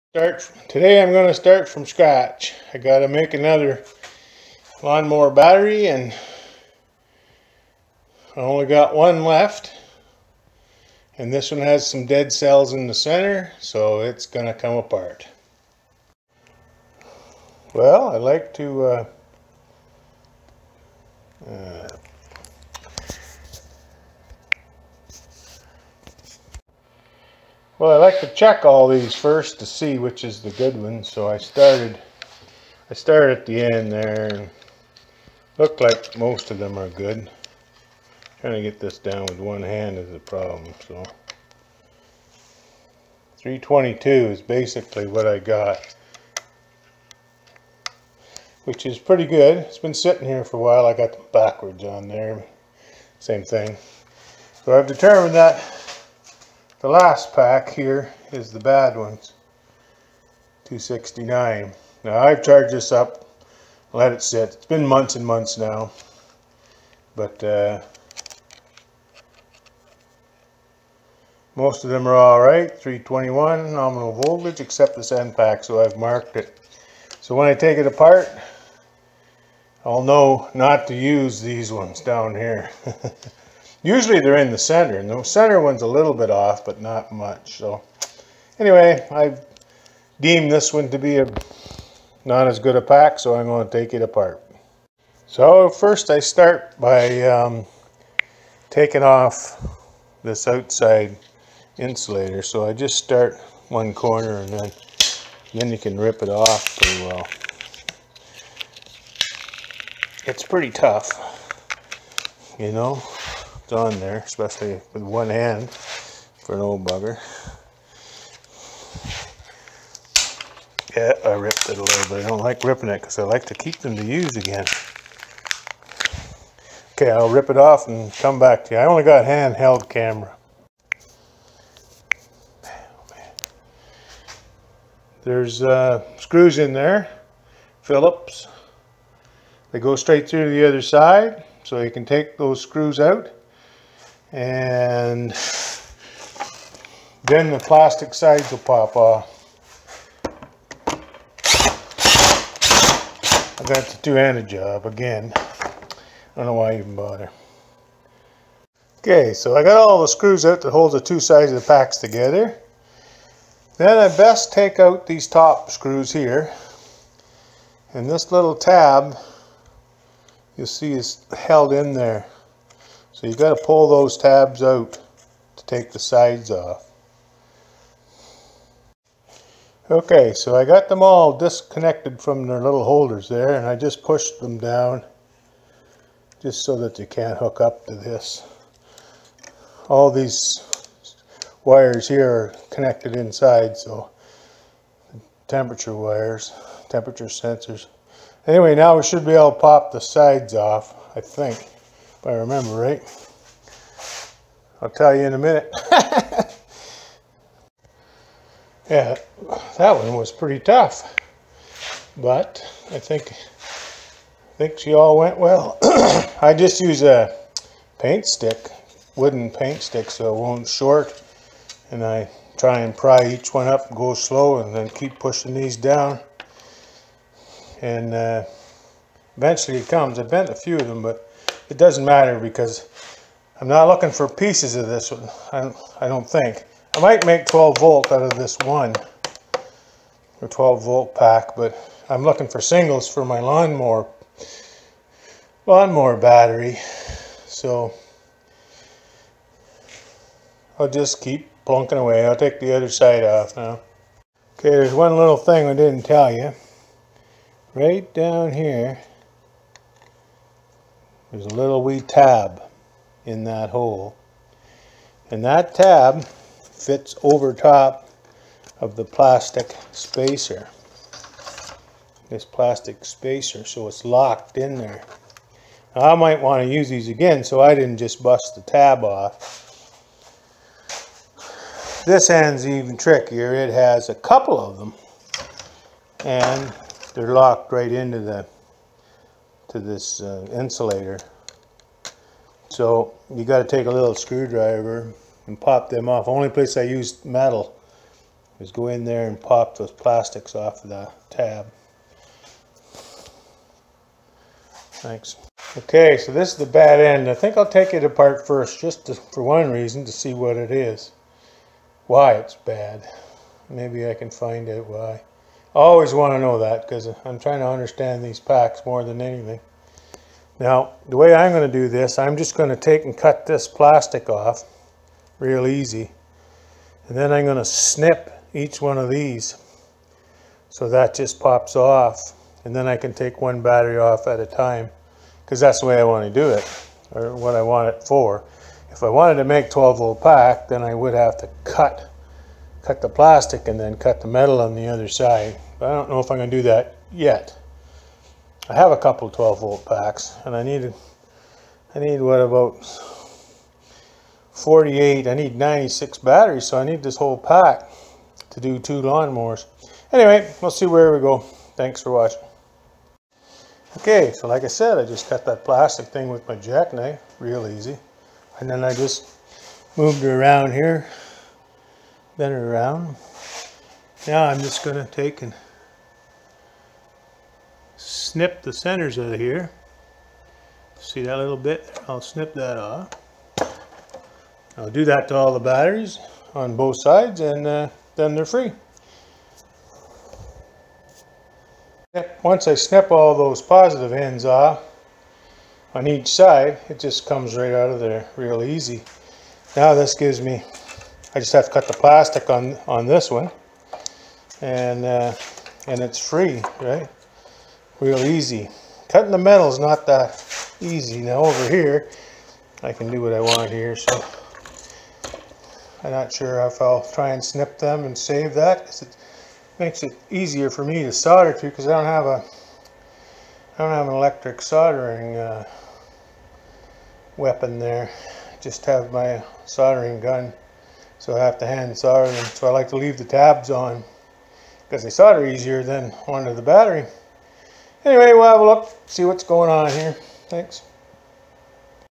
2020 21 A123 battery pack dismantle
Whatever I do is for my learning experience and not recommended. I need another lawn mower battery so I am dismantling an A123 battery pack.